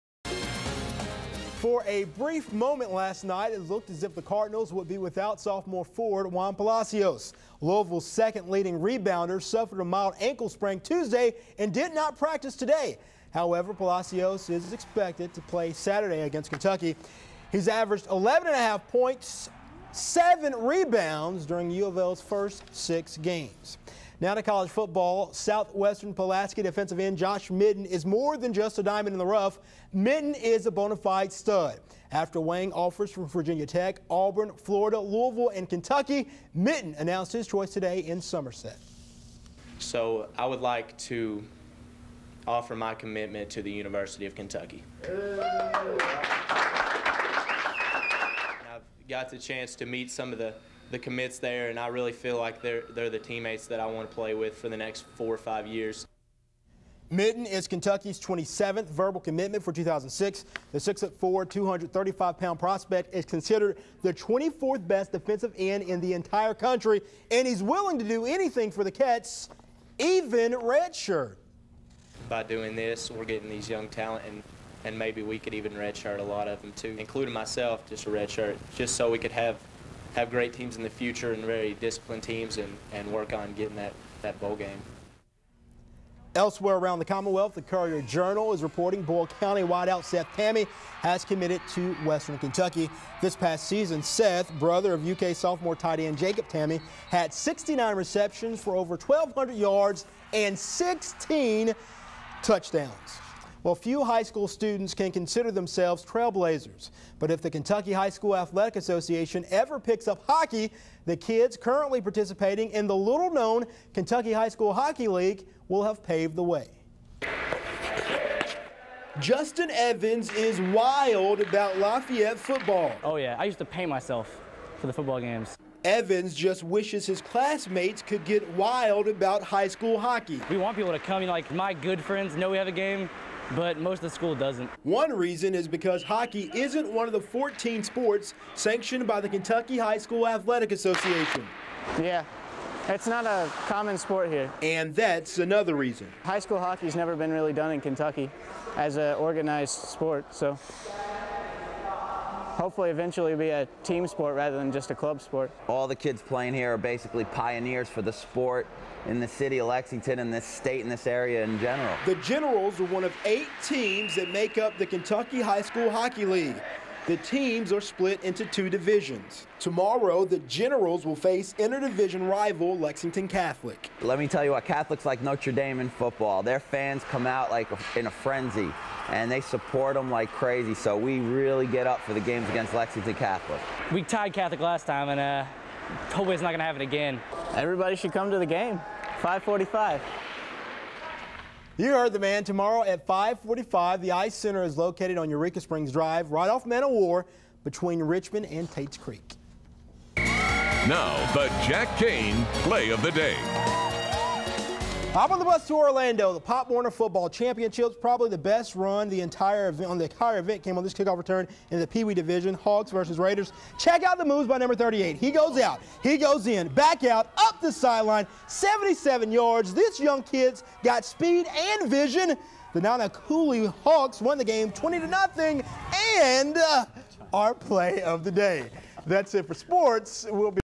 Sportscast -hoops-fball-and prep hockey pkg
Sportscast from 2006 including prep hockey league